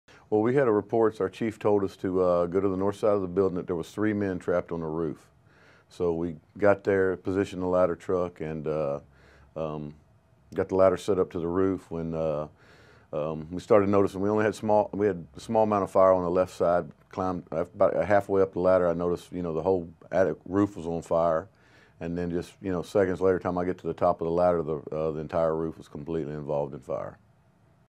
TX FIRE RESCUE VIDEO FIREFIGHTER-SCENE
The firefighter involved in the dramatic rescue of a construction worker from a burning building talks to CNN.